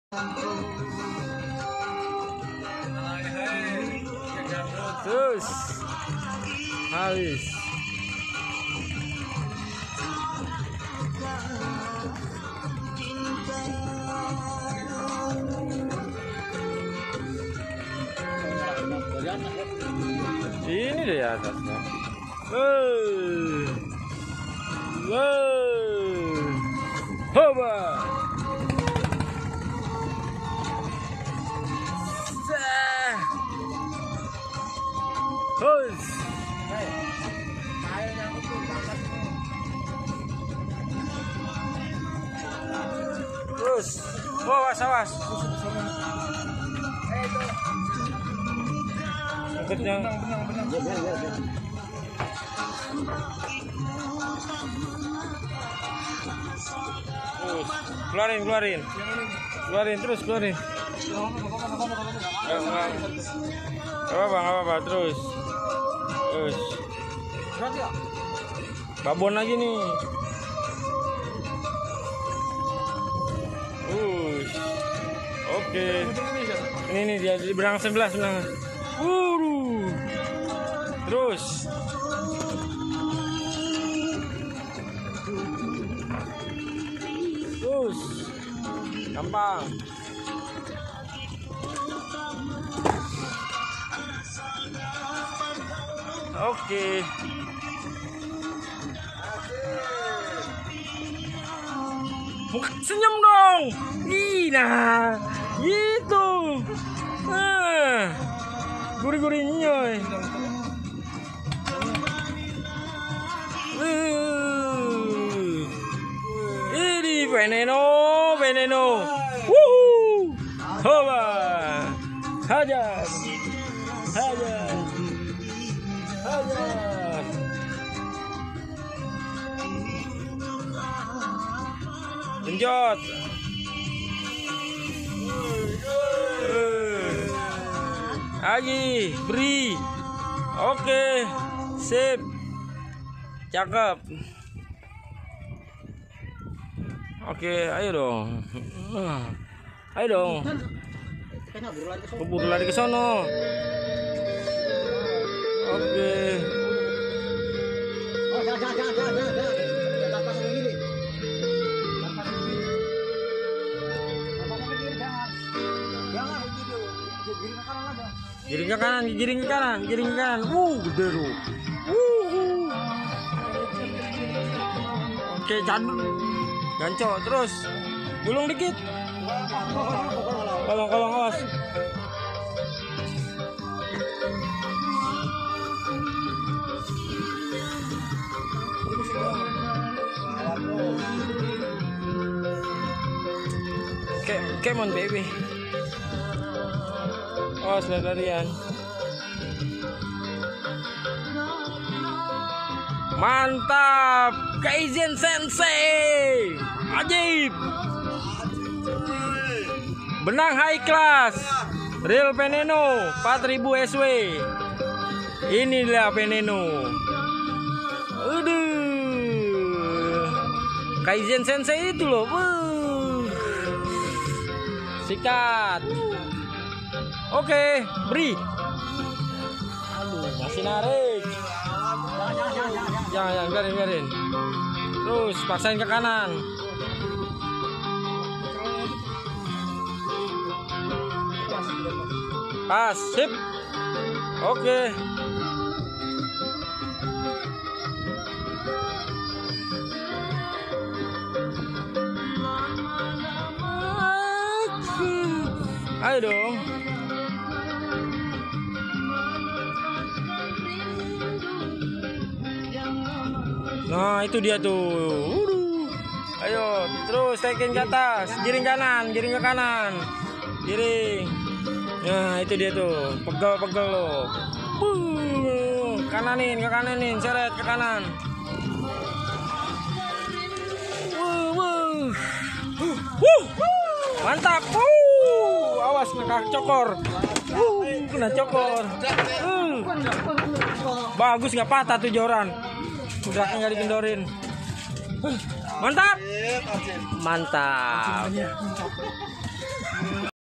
Reel Kamikaze Veneno 4000SW - Strike Tengiri Pulau Damar
Trip 24 November 2019
Bersama rekan Sahabat MDM , Akay Delpiero, Yanto Cahndeso, Agus RG

Target          : Tengiri (spannish mackerel)
Umpan.        : Tembang / Kembung (chub mackerel)
Rod/Joran.  : Kaizen Sensei PE 1-3
Reel.             : Team Kamikaze Veneno 4000SW
Mono line.   : Hi-Class 0.29 Lb. 18Kg
Spot              : Edam Islands / P.Damar Kep.1000 JKT
Province      : Special Region of Jakarta
Nation          : Rep. Of Indonesia 🇮🇩
Boat Capt.   : Sasmita (PLTU Ancol - Bahtera)
Mobile Cam: Vivo V15 PRO Camera